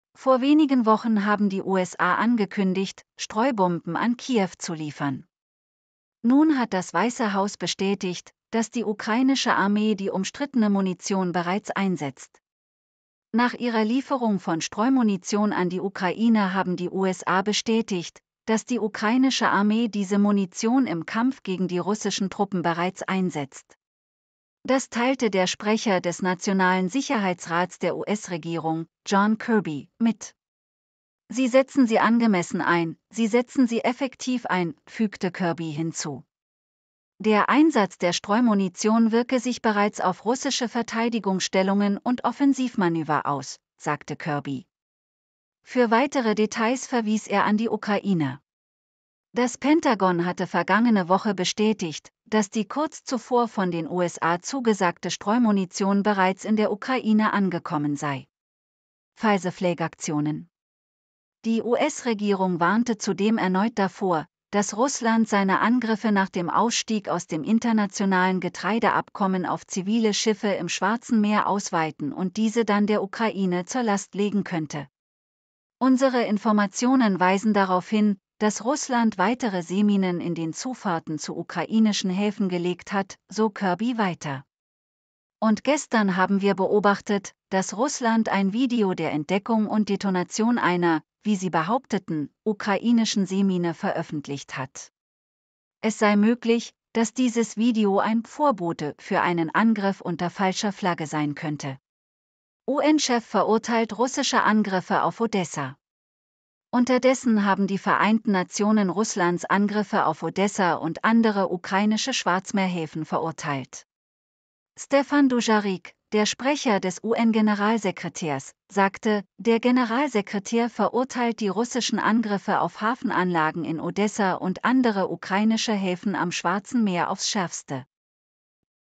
USA bestätigen Einsatz von Streumunition durch Ukraine
Nach ihrer Lieferung von Streumunition an die Ukraine haben die USA bestätigt, dass die ukrainische Armee diese Munition im Kampf gegen die russischen Truppen bereits einsetzt. Das teilte der Sprecher des Nationalen Sicherheitsrats der US-Regierung, John Kirby, mit.
Infos, News & Zeitgeschehen - Internationale Live-Informationen.